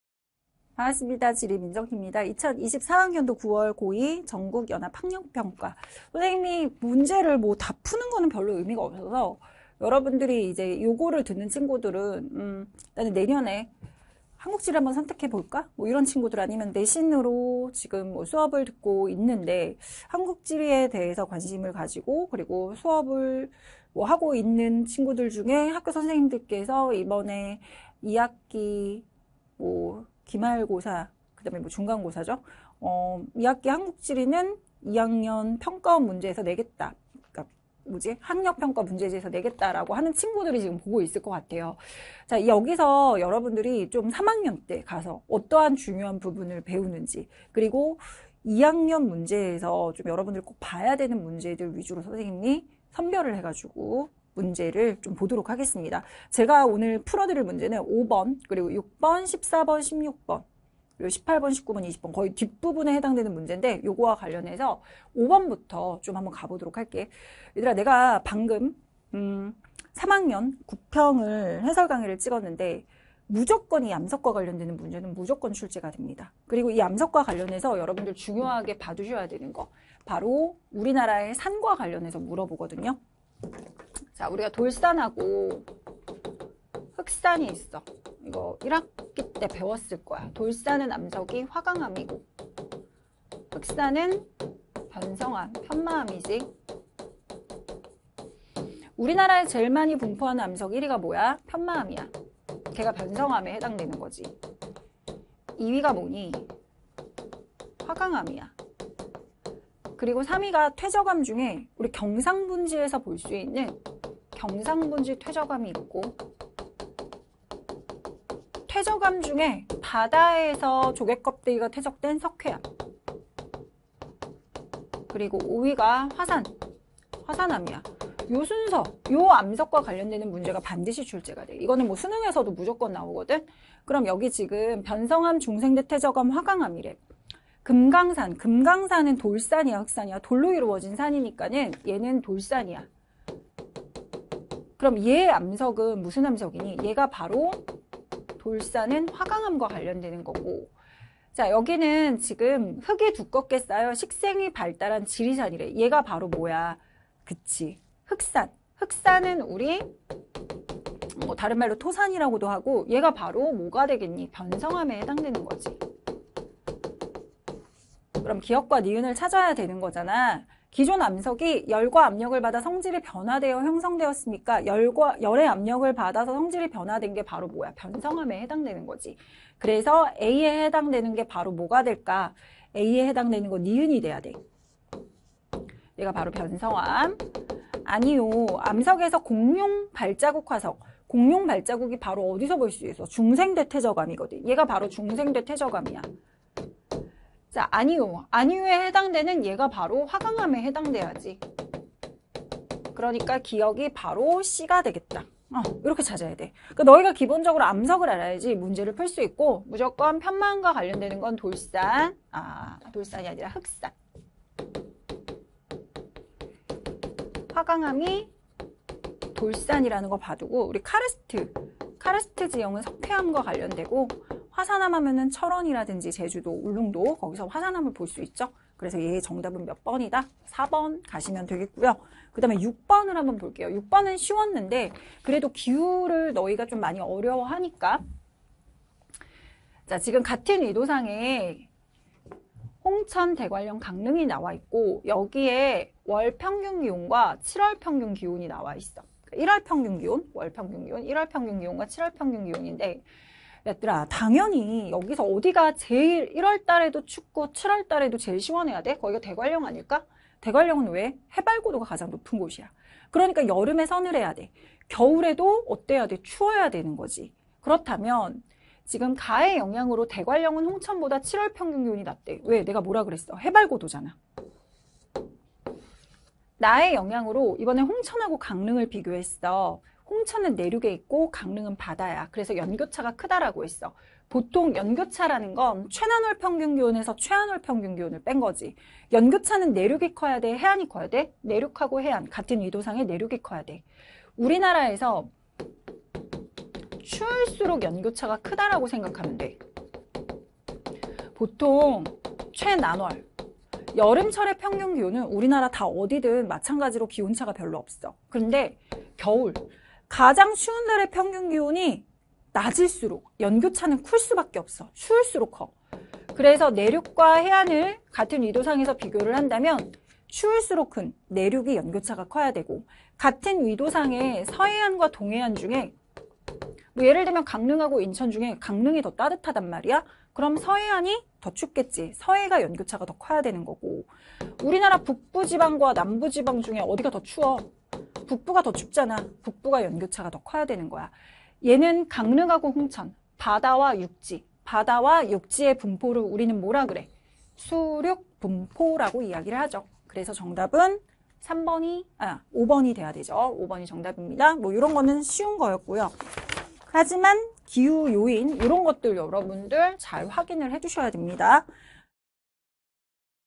[메가스터디] 사회 민정쌤 - 2024년 고2 9월 모의고사 한국지리 해설강의
사회 민정 선생님의 2024년 고2 9월 모의고사 한국지리 해설강의 영상입니다.

- 사회 민정 선생님을 더 알고 싶다면?